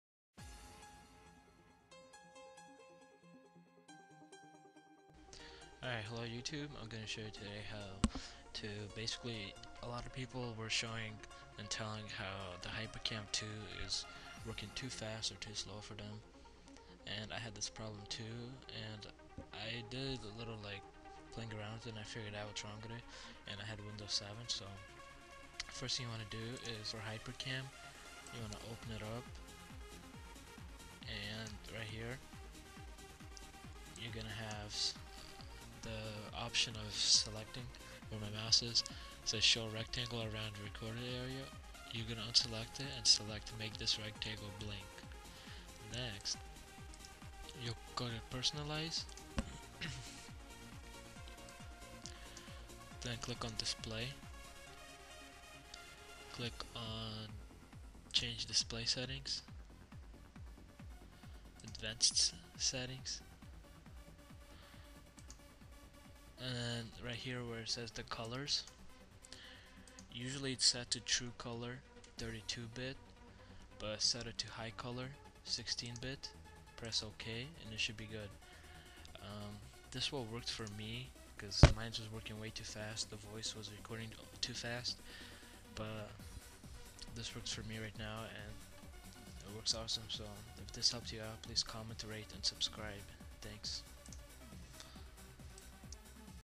HyperCam 2 FIX Windows 7
Having problems with HyperCam2? is it recording too fast? Watch my video for the fix!  This is for windows 7 and vista